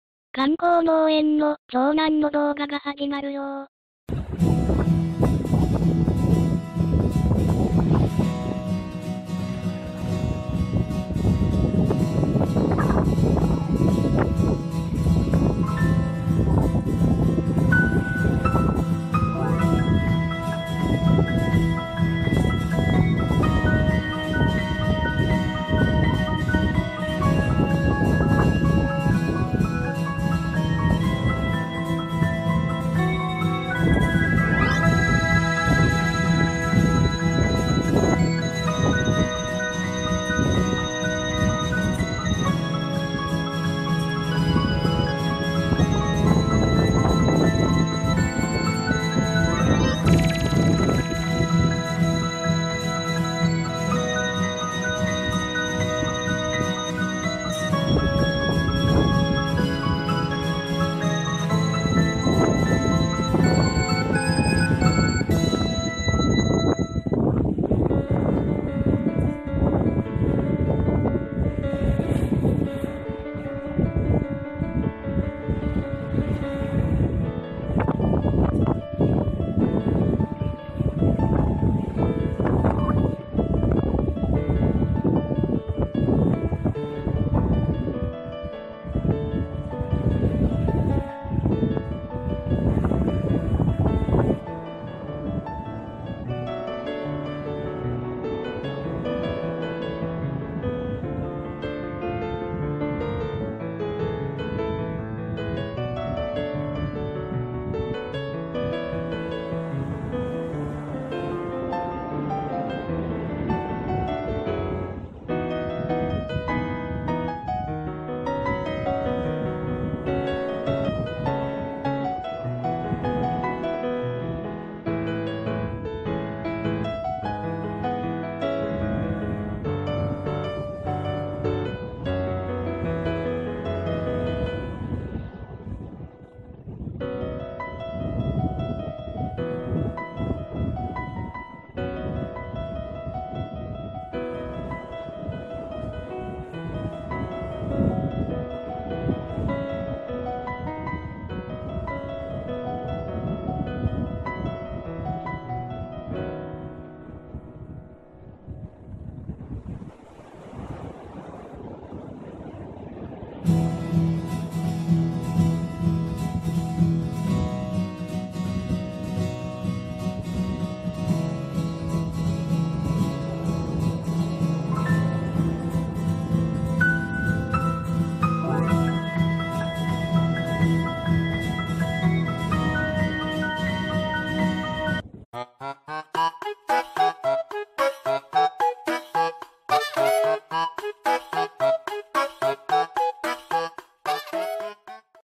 【エギング&サーフ】観光農園の長男釣りに行く！
釣りよか見てたら釣りに行きたくなって、観光農園の長男がエギングにチャレンジが・・・撃沈(*ToT)

本家釣りよかでしょうのエギング動画